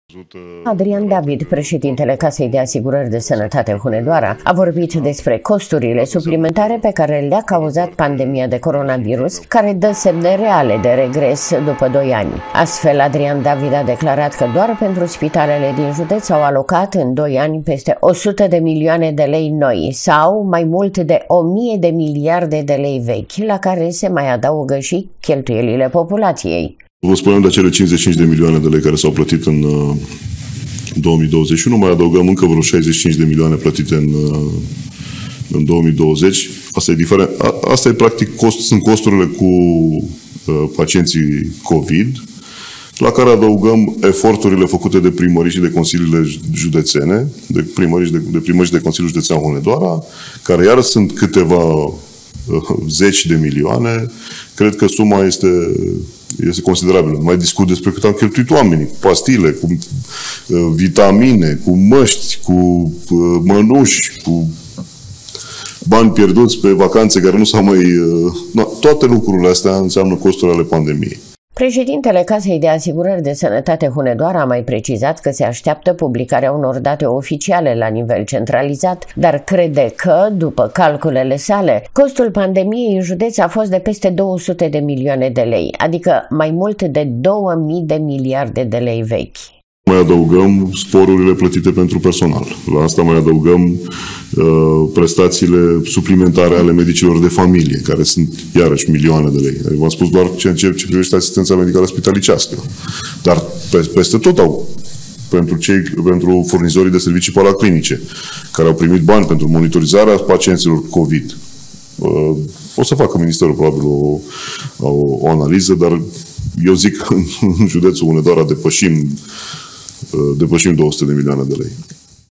PANDEMIA A COSTAT JUDEȚUL HUNEDOARA MAI MULT DE DOUĂ MII DE MILIARDE DE LEI VECHI
Pandemia de coronavirus pare să fi fost învinsă, dar a lăsat în urma ei, pe lângă pierderi de vieți omenești și suferință, și cheltuieli imense. Oficialii din sănătate spun că la nivelul județului Hunedoara acestea ating sume amețitoare.